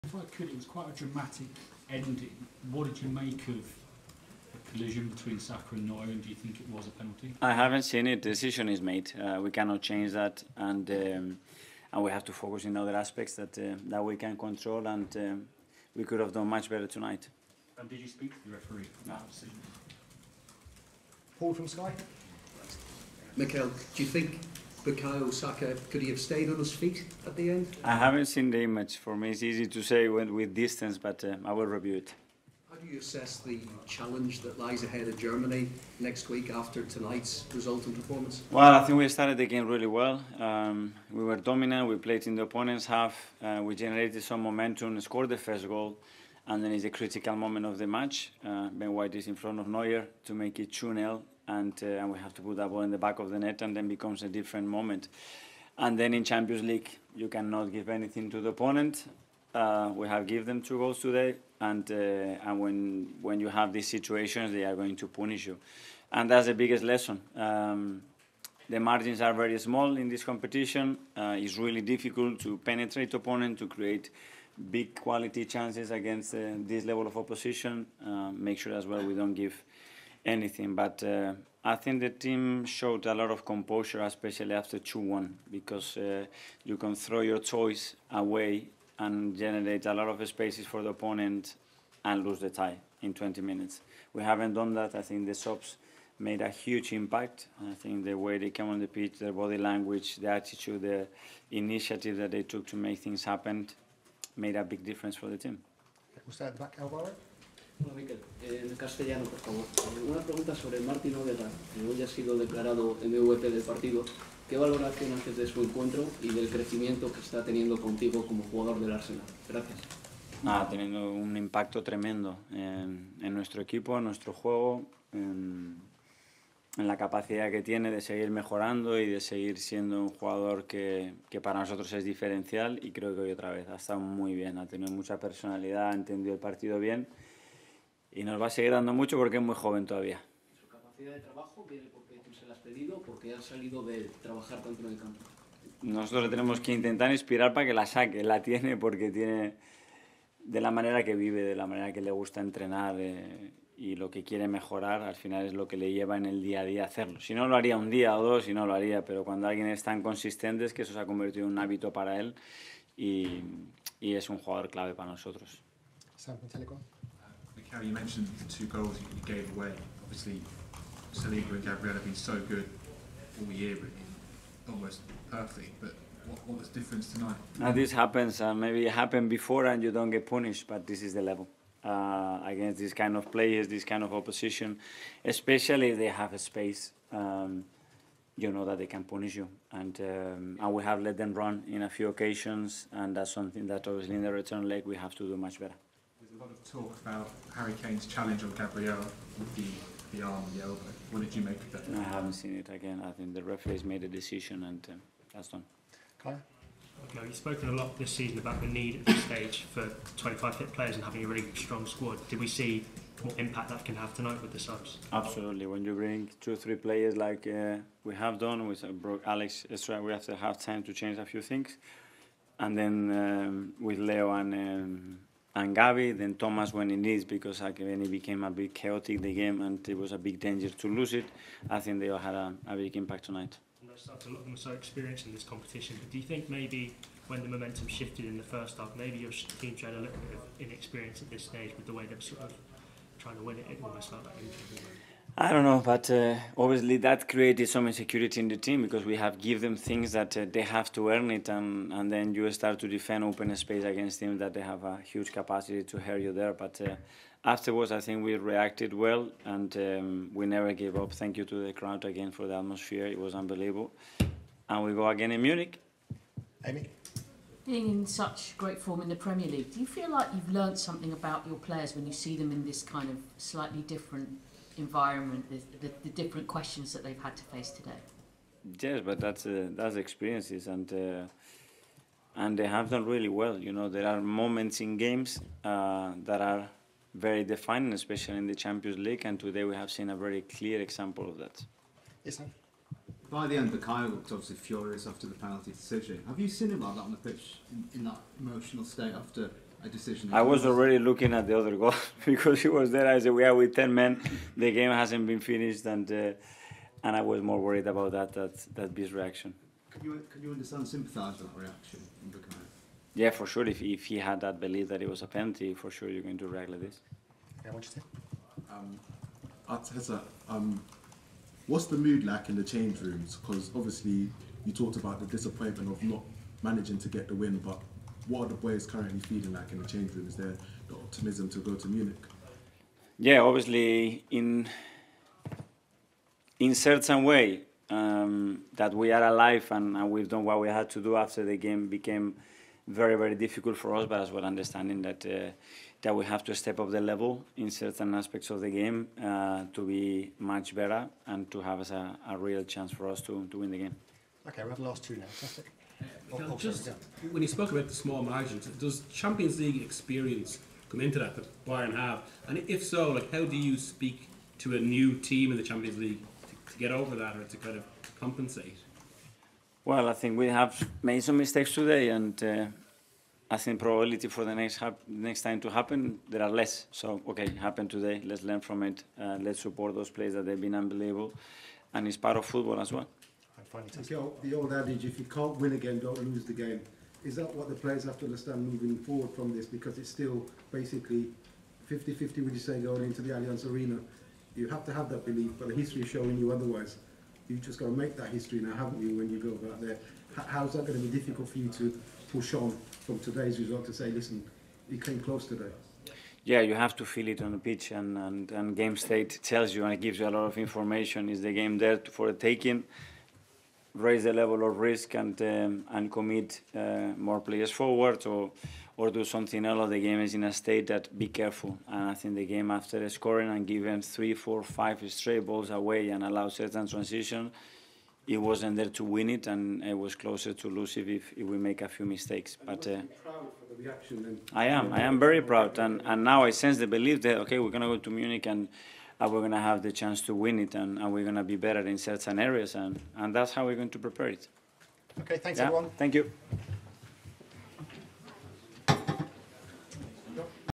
WE GAVE THEM TWO GOALS! | Mikel Arteta | Arsenal 2-2 Bayern Munich
-TIMESTAMPS-

00:00 - Intro.

00:08 - Saka pen?: I haven’t seen it. We could’ve done much better.

00:29 - Could saka have stayed on his feet: I haven’t seen it

00:53 - In champions league we can’t give anything. Today we gave Bayern two goals. The subs made a huge impact.

02:02- Spanish answer.

02:27 - Spanish answer.

02:57 - Saliba and Gabriel: This happens. This is the level. We let them run through and we need to do better.

03:21 - Harry Kane elbow: I haven’t seen it.

03:36  - Need for 25 fit players and the impact: When you bring 3/4 players on like we did. They had a big impact.

04:24 - Immaturity at this stage?: It created some insecurity in the team. We reacted well and never gave up. We go again in Munich.

04:38 - Learnt something about your players?: That’s the experiences, they’ve done so well.

05:00- Saka angry: I was already looking at the other goal, the game wasn’t finished!

05:14 - Sympathise with him?: If he thinks it’s a penalty for sure you’ll react like this.

05:34 - optimistic to go to Munich?: We have to step up the level to be much better and a real chance to win.

06:09 - Champions league experience?: We made mistakes today, let’s learn and support.

06:46 - You have to feel it on the pitch, it was closer to lose. I am proud by the reaction.